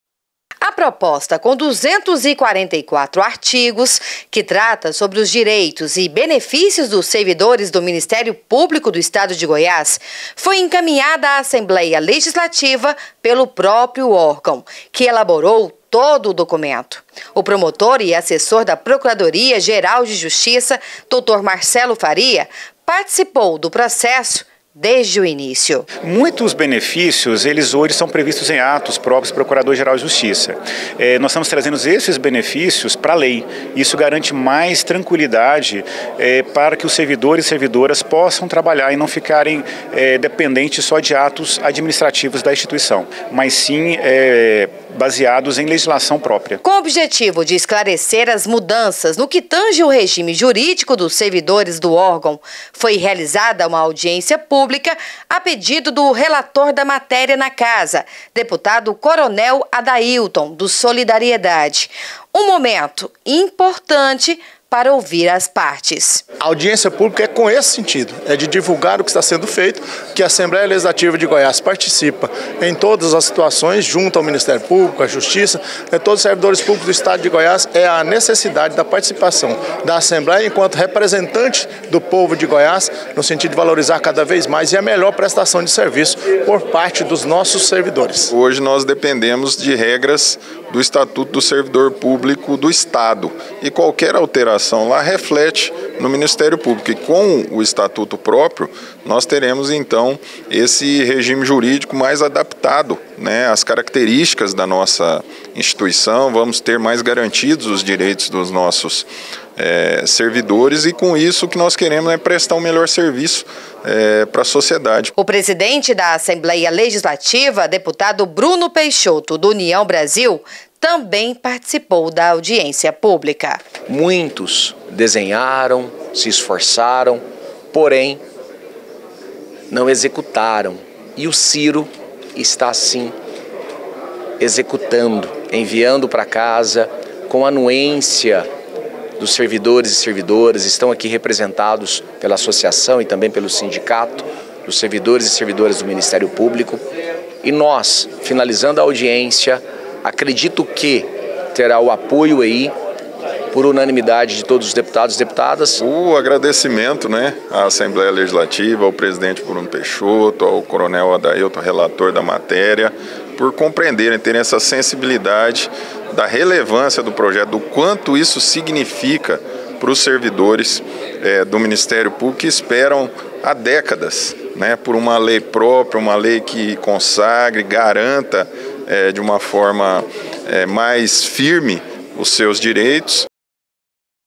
Alego News: Audiência Pública Estatuto do Ministério Público (20 /08/2024)
A proposta com 244 artigos que trata sobre os direitos e benefícios dos servidores do Ministério Público/GO foi encaminhada a Assembleia Legislativa pelo próprio órgão, que elaborou todo o documento. O promotor e Assessor da Procuradoria Geral de Justiça, doutor Marcelo Faria participou do processo desde o início:
Doutor Marcelo Faria - Promotor de justiça e Assessor da Procuradoria Geral de Justiça

Com o objetivo de esclarecer as mudanças no que tange o regime jurídico dos servidores do órgão, foi realizada uma audiência pública a pedido do relator da matéria na Casa, dep. Coronel Adailton (Solidariedade). Um momento importante para ouvir as partes
Dep. Coronel Adailton (SD) -Relator do Estatuto do Ministério Público
Doutor Cyro Terra Peres/ Procurador Geral de Justiça.

O presidente da Assembleia Legislativa, dep. Bruno Peixoto (UB) também participou da audiência pública:
Dep. Bruno Peixoto(UB) - Presidente da Casa

Destaques e entrevistas na reportagem de Patrícia Lee e equipe TV Assembleia de Goiás.